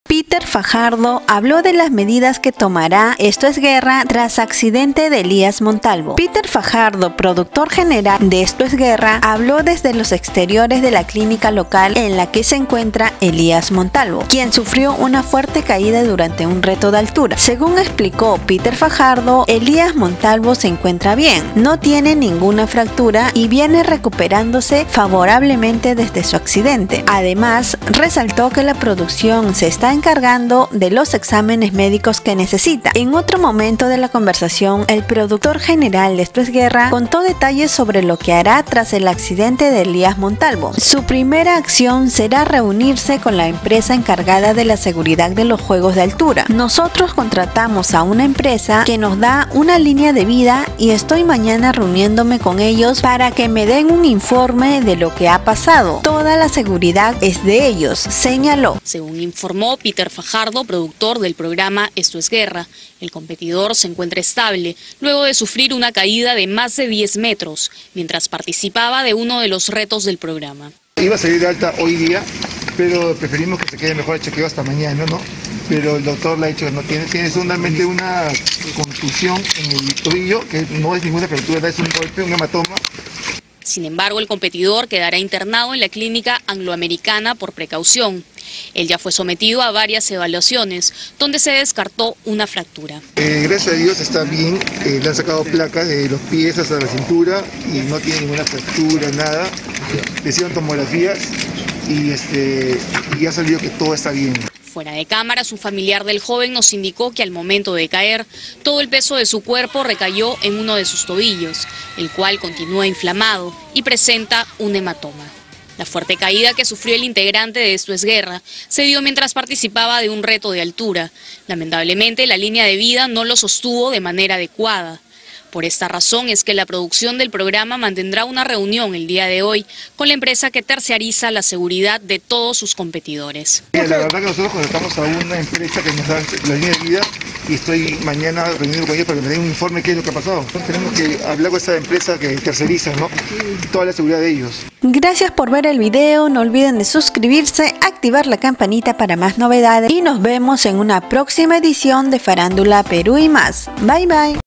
¡PETER FAJARDO, PRODUCTOR DE EEG NO RECONOCE SU CULPA POR CAIDA DE ELIAS MONTALVO! MINIMIZA TRAGEDI4
¡SE LAVA LAS MANOS! PETER FAJARDO DESCONOCE Y SE EXCULPA DE LA CAIDA SUFRIDA POR ELIAS MONTALVO EN EEG! PRODUCTO DE EEG MINIMIZA ACCIDENTE EN RETO DE ALTURA!!